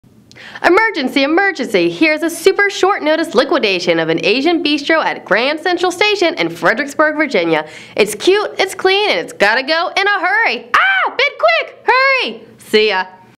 Asian Bistro Auction
Emergency Emergency! Hers a super short notice liquidation of this asian bistro at Grand Central Station in Fredericks burg. Its cute, its clean and its gotta go in a hurry!!! Aghhh bid quick, hurry! See Ya